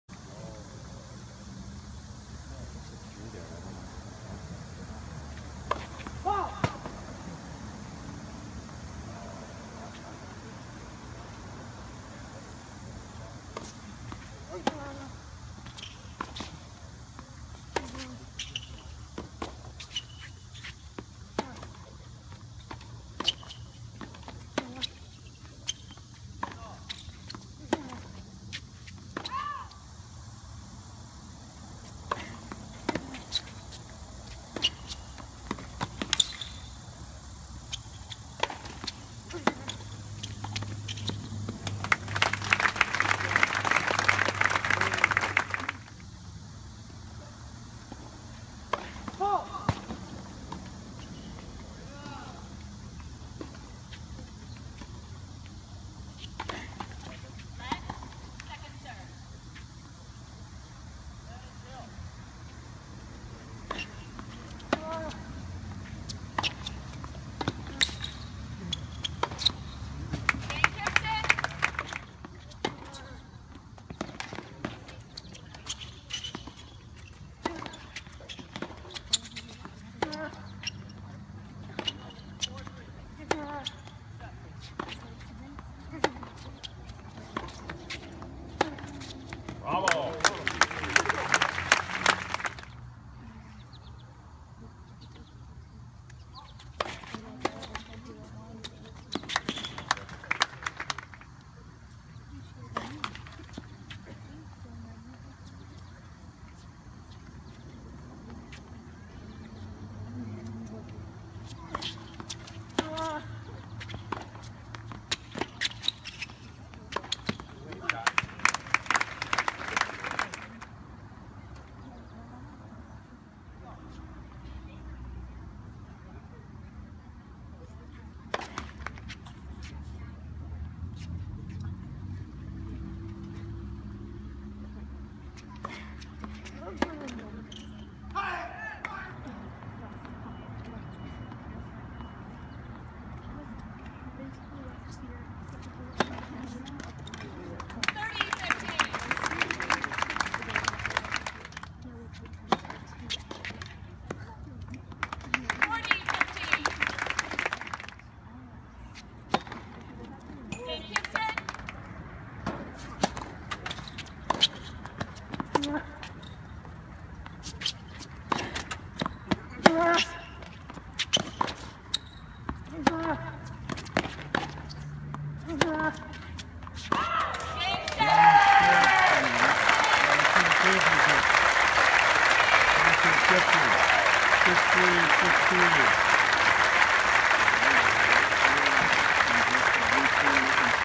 Kypson 16s Final Kalamazoo
Patrick Kypson in Kalamazoo Nationals 16s final, August 9, 2015, Stowe Stadium, Kalamazoo College. Kypson defeated Alexandre Rotsaert 6-3, 6-3